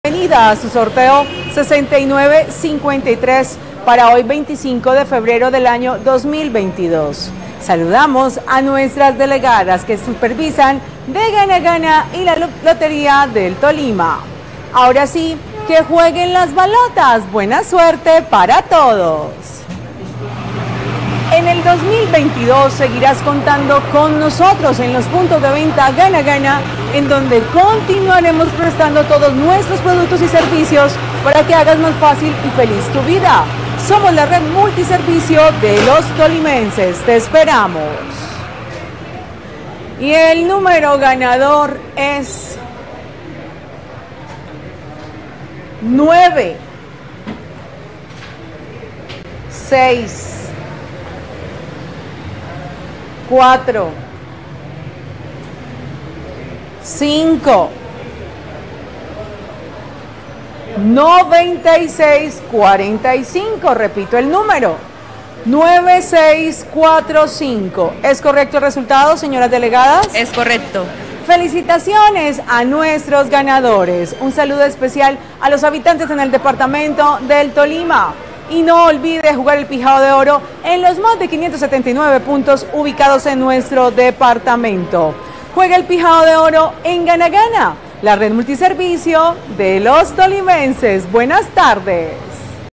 PIJAO DE ORO Resultado del VIERNES 25 de febrero del 2022 ✅🥇🔥💰
Hola buenas tardes, te presentamos los resultados de las loterías y chances en Colombia, vamos con el Resultados del Chance Pijao de Oro del VIERNES 25 de febrero de 2022.